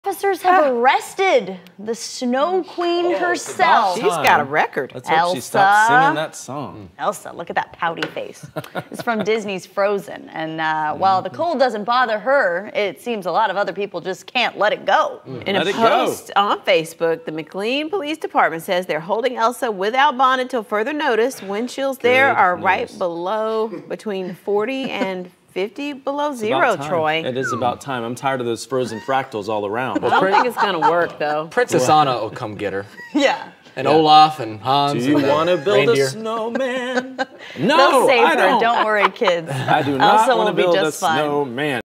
Police arrest Elsa for freezing weather
McClean Police Department arrest Disney's snow queen for the freezing weather.